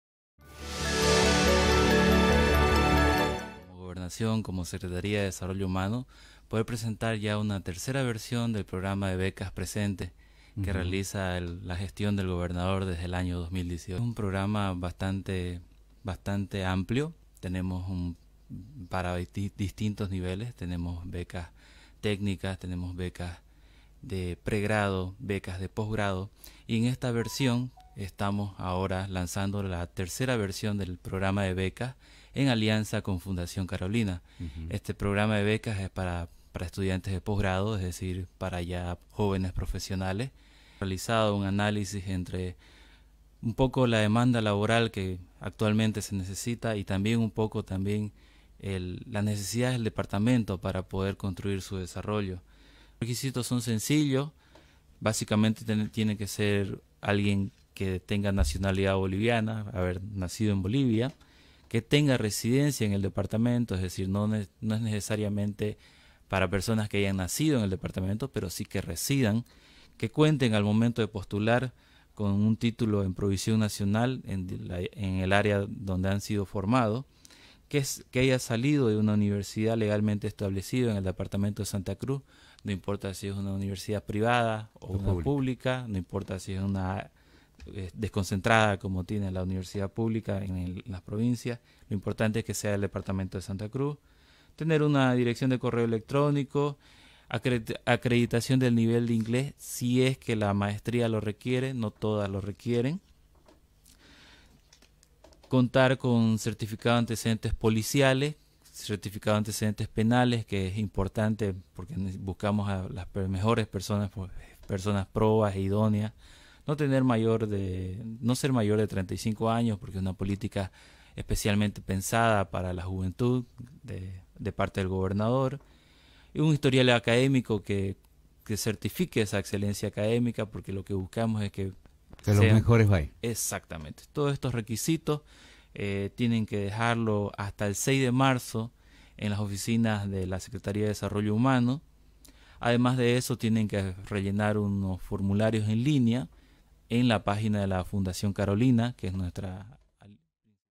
Becas de la Gobernación oportunidad para profesionales de Santa Cruz – EL DEBER RADIO
Está abierta la convocatoria para las becas “Una oportunidad para tu futuro”, el Gobierno Autónomo Departamental de Santa Cruz busca dar apoyo a la formación del conocimiento, con el objetivo de fortalecer la investigación para el desarrollo socio-productivo de la región.

Este año junto con la fundación Carolina se logró publicar quince becas en universidades españolas de manera presencial.
Esta oportunidad está dirigida a jóvenes profesionales que buscan continuar con su formación académica.

Daniel Limpias responsable del proyecto mencionó los requisitos para postular son: Ser de nacionalidad boliviana con residencia en Santa Cruz, contar con el título profesional, tener una dirección de correo electrónico, contar con certificado de antecedentes penales, no ser mayor de 35 años e historial académico que garantice la excelencia del solicitante.